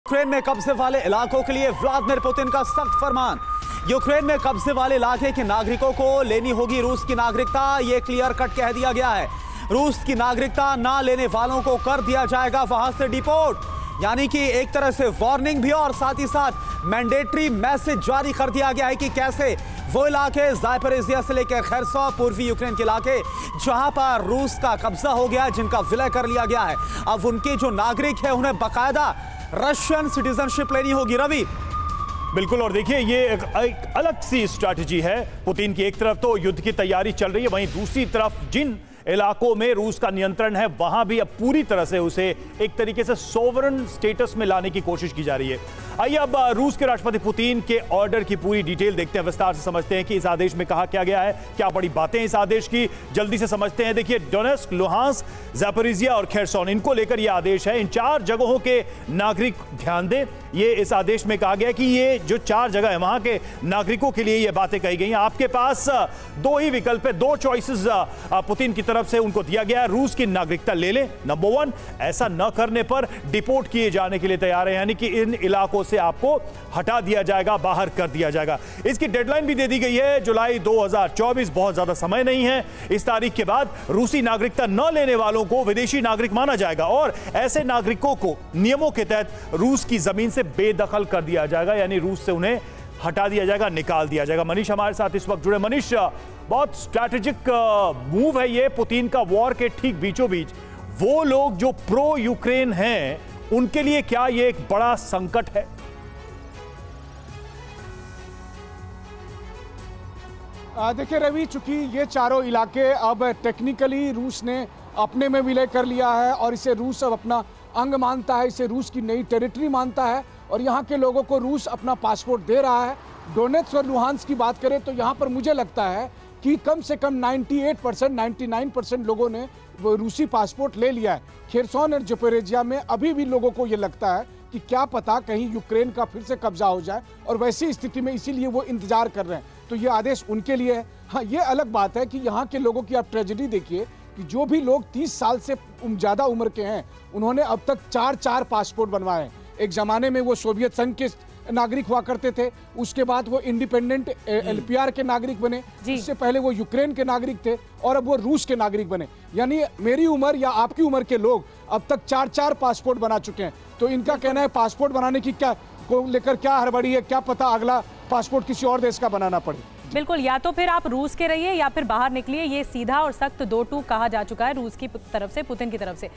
Russia-Ukraine Breaking : यूक्रेन में कब्जे वाले इलाकों के लिए Putin का सख्त फरमान
यूक्रेन में कब्जे वाले इलाकों के लिए पुतिन का सख्त फरमान, यूक्रेन में कब्जे वाले इलाके के नागरिकों को लेनी होगी रूस की नागरिकता